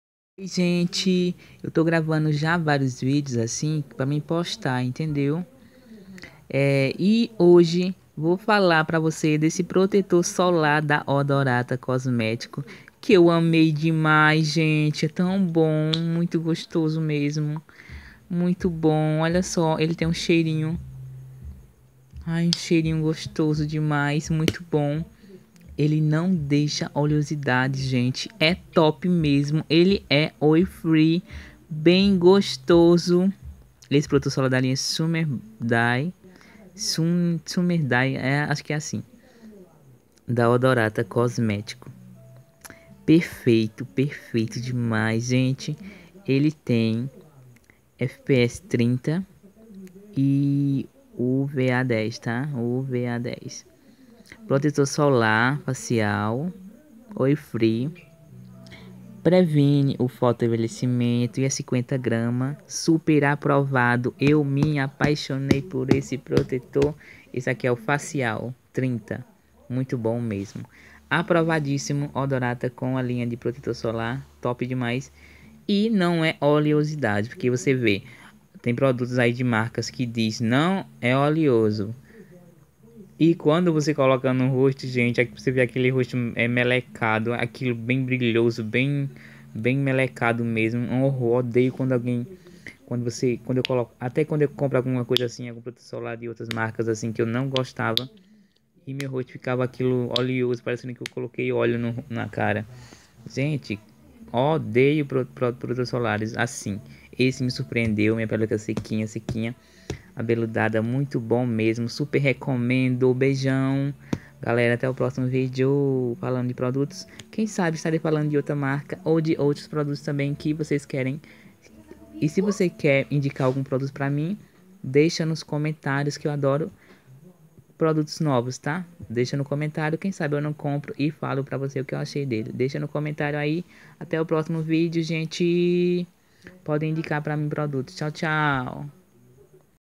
Falando de cosméticos
O que achei  desde protetor solar da adorata cosméticos.